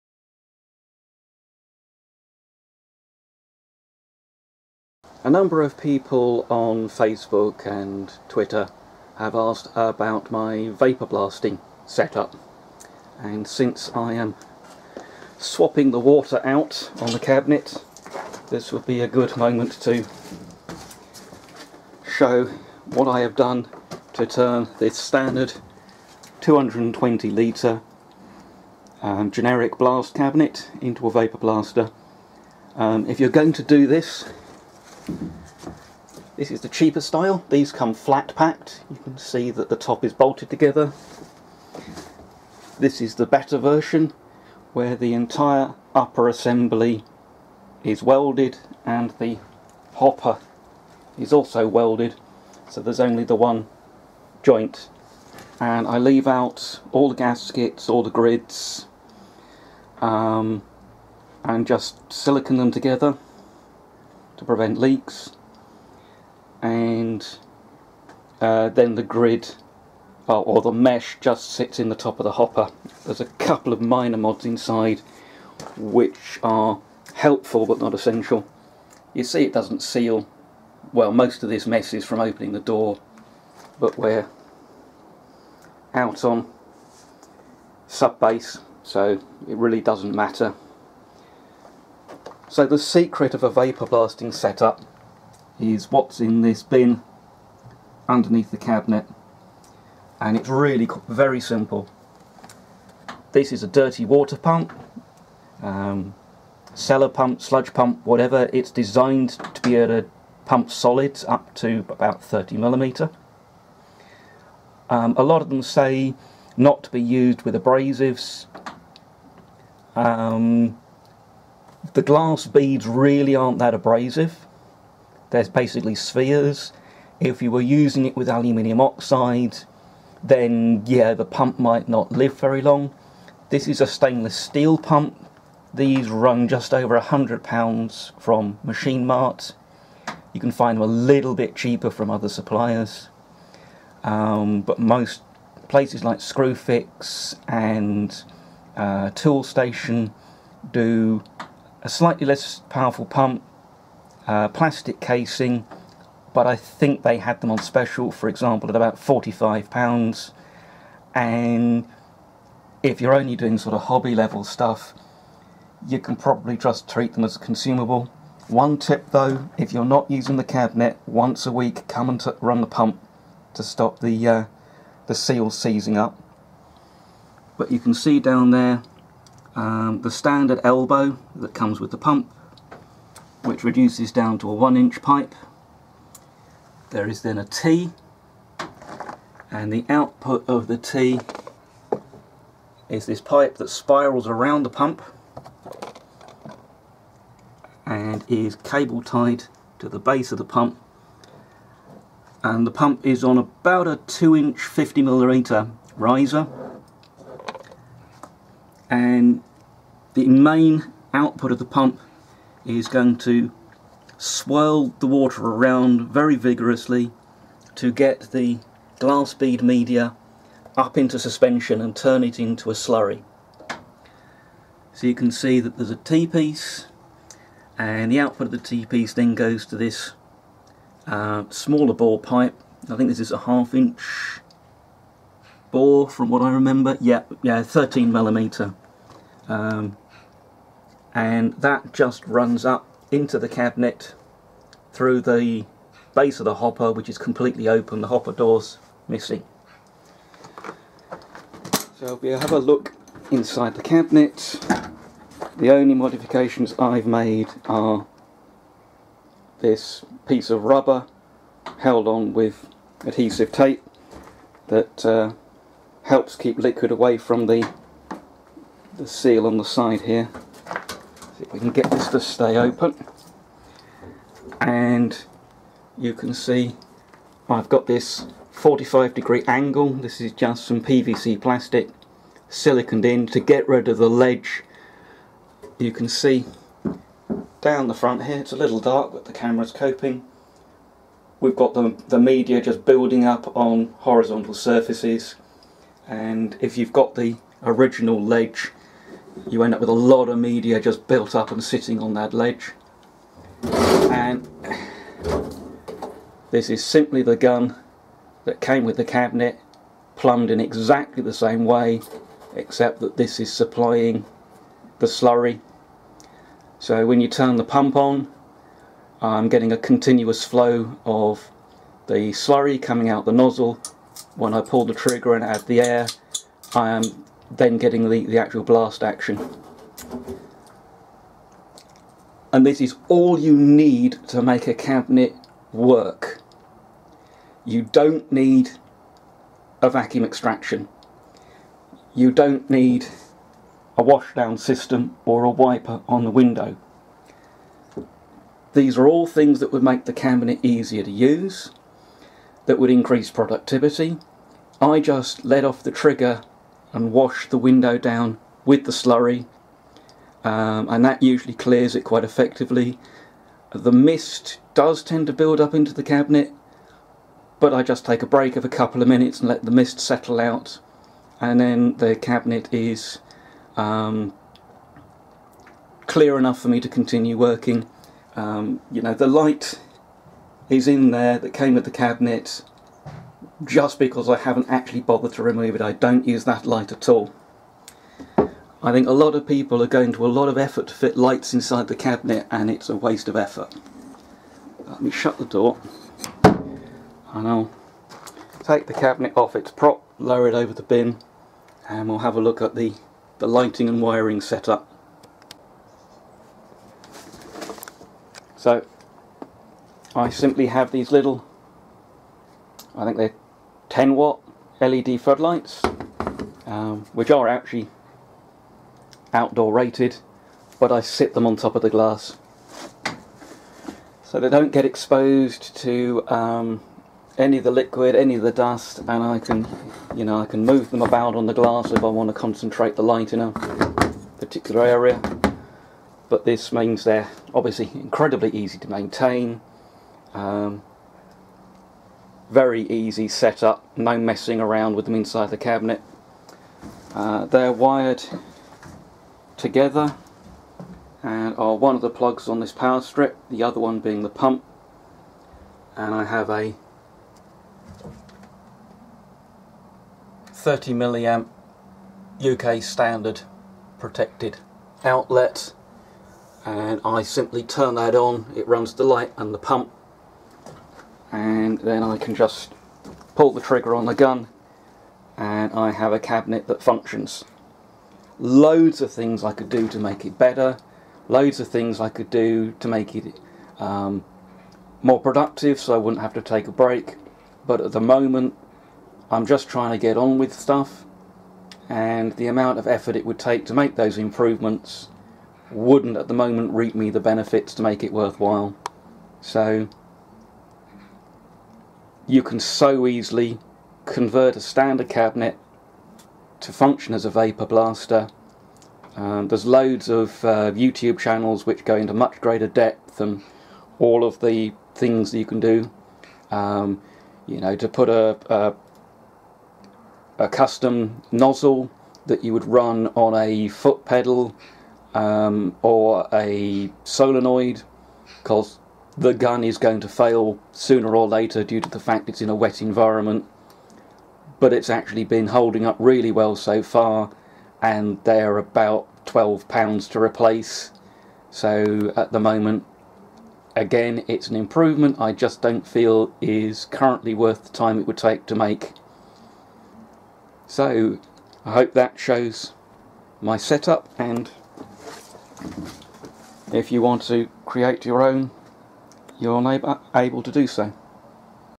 A simple DIY Vapor Blaster
Converting a standard blast cabinet into a vapor blaster need not be complex. The only thing you NEED is a way of generating the slurry to feed to the gun.